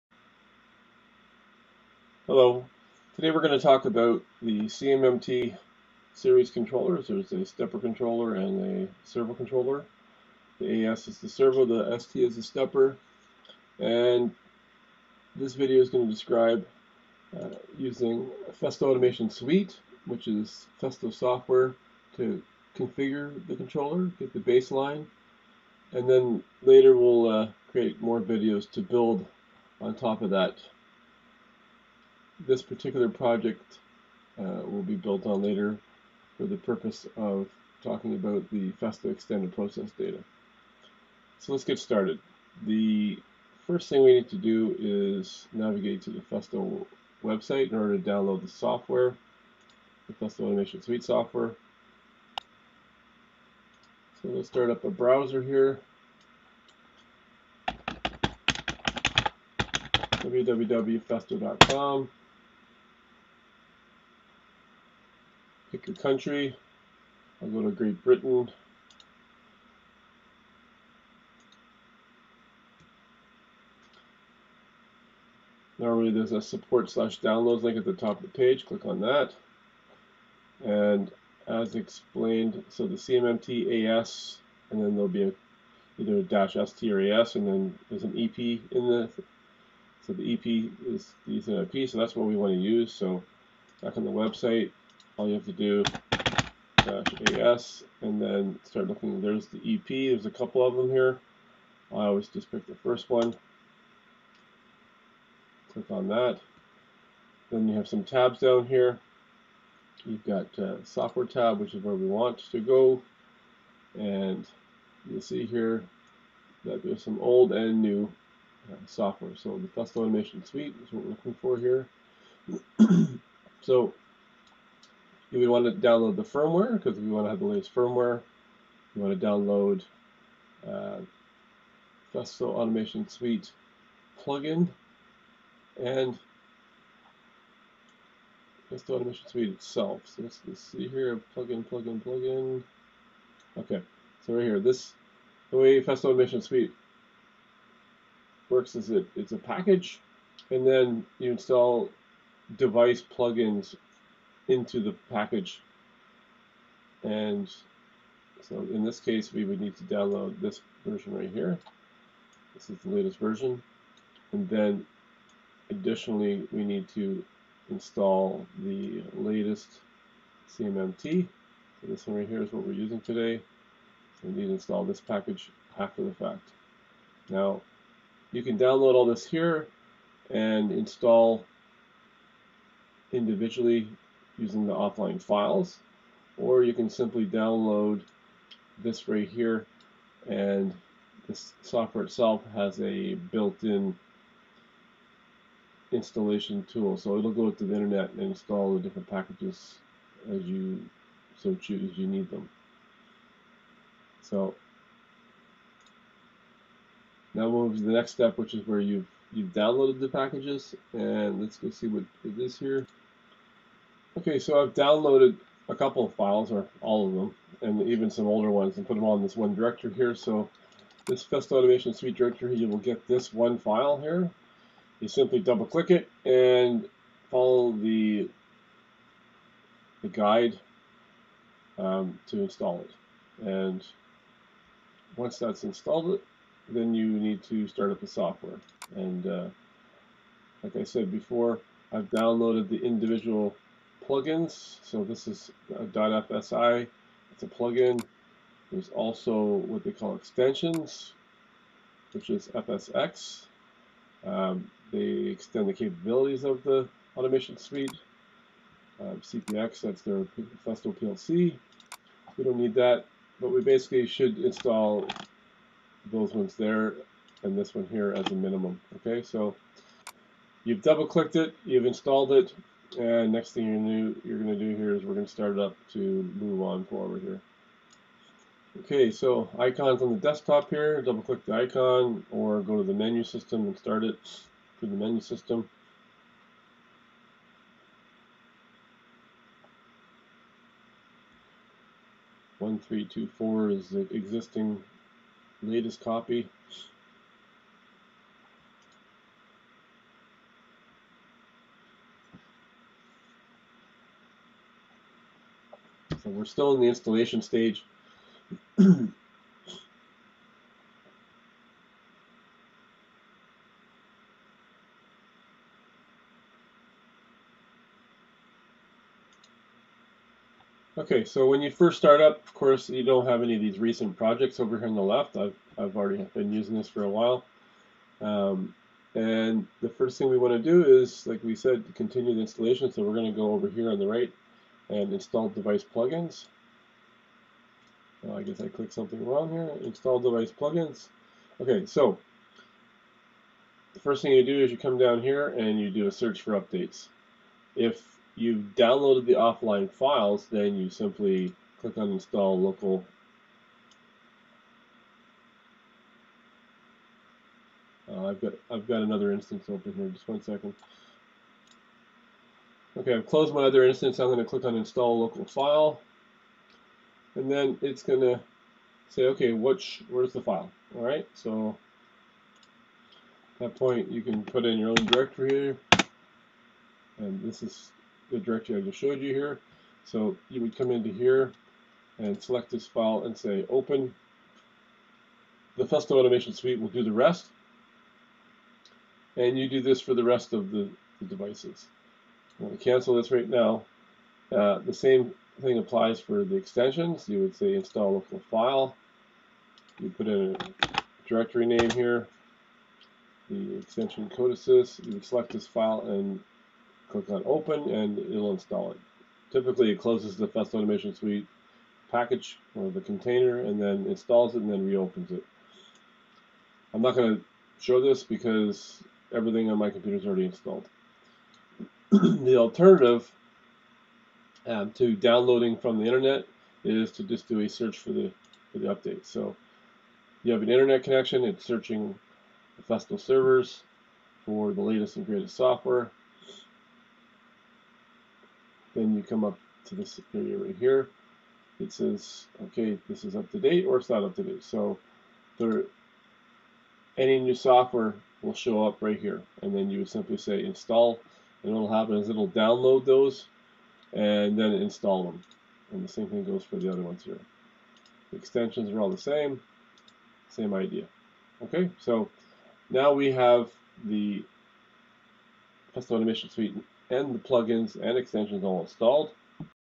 CMMT-xx-EP x1 + ControlLogix + EthIP + Tel_910 + EPD + Step1 + FAS Download and Installation
Check out the link to sample code below!!

Festo Automation Suite (FAS) is a multipurpose software tool.  One purpose is to configure and commission a CMMT series controller.
This video describes the process to download and then install the software and plug-ins and extensions.

(0:01) Overview of Video content
(0:47) Download Festo Automation Suite (FAS) software from Festo website
(4:38) FAS start the software and continue installation of Plug-ins and Extensions

There are some files (within a “zip” file) to accompany this video that can be retrieved at the following Google Drive link: